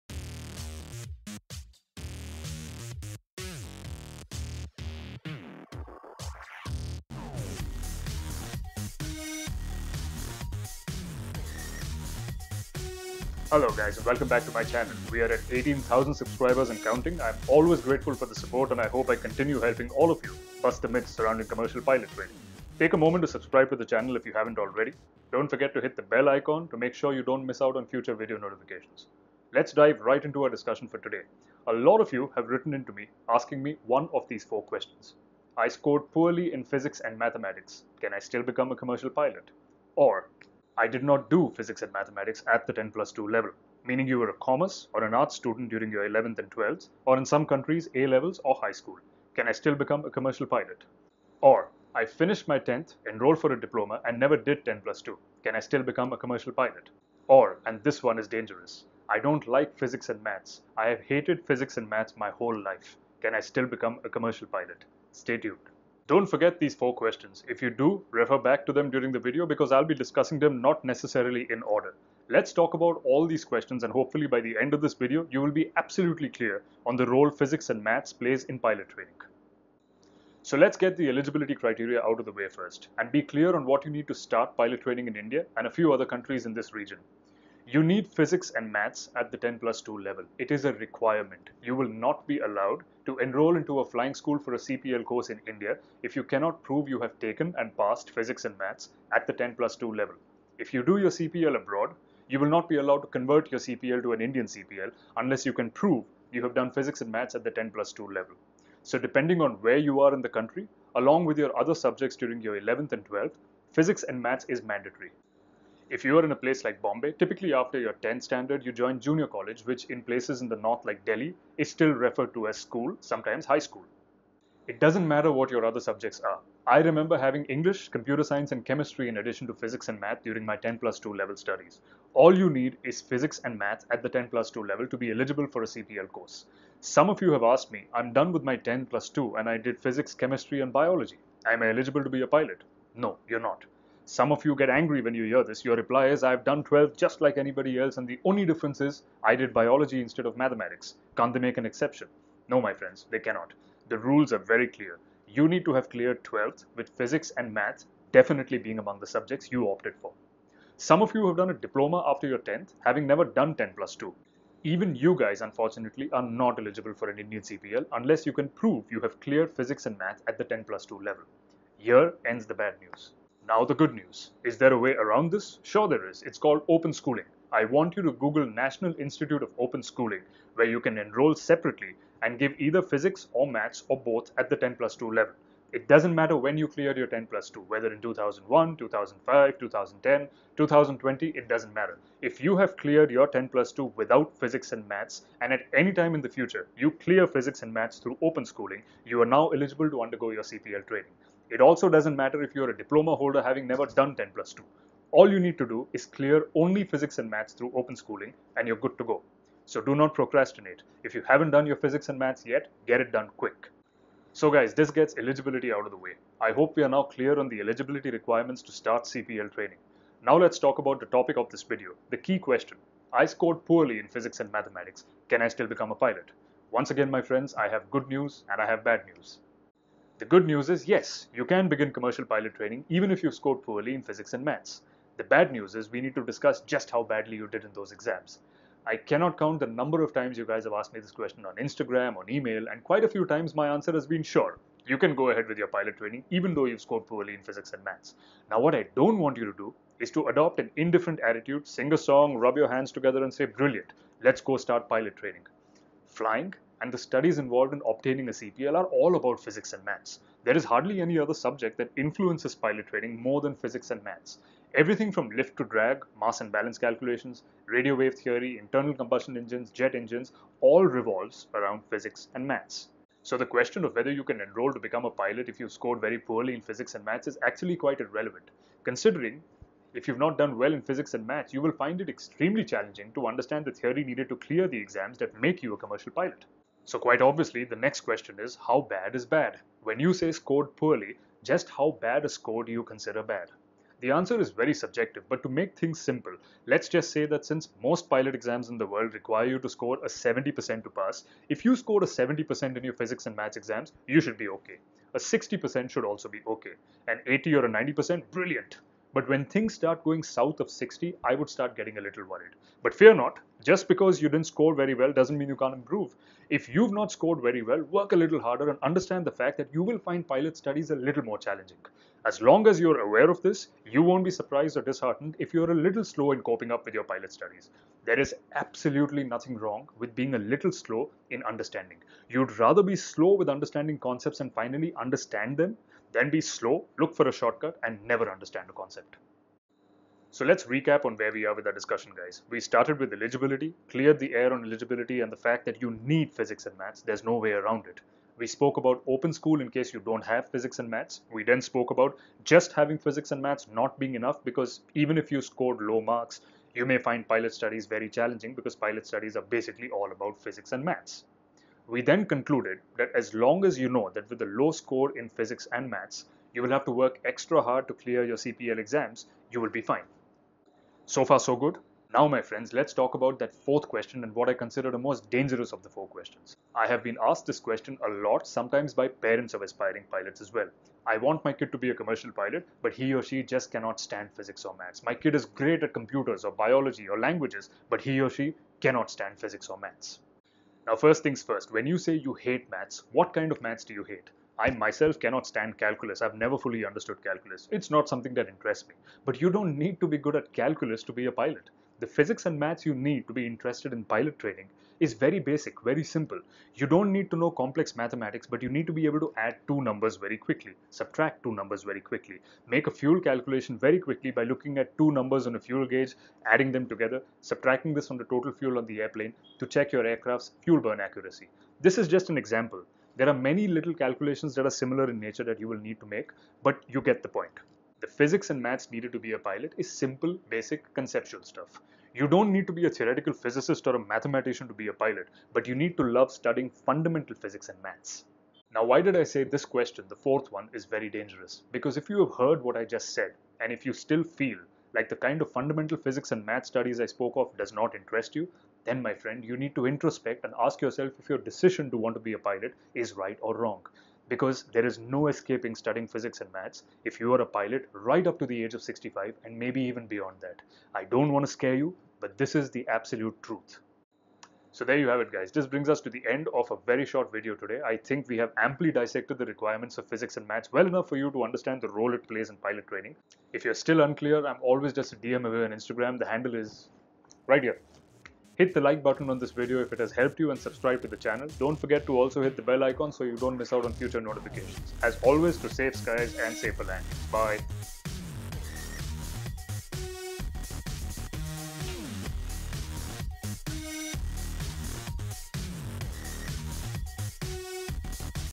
My Physics and Maths scores are low, can I still become a Commercial Pilot-Answered by Samarth Singh
Did you score less than expected in your Physics and Maths exams at the 10+2 level? Wondering if it will affect your pilot training or eligibility. Let's discuss the role of Physics and Maths in a DGCA India CPL.

DM me your queries on Instagram or Twitter - @captsamarth - Capt. Samarth Singh Wadalia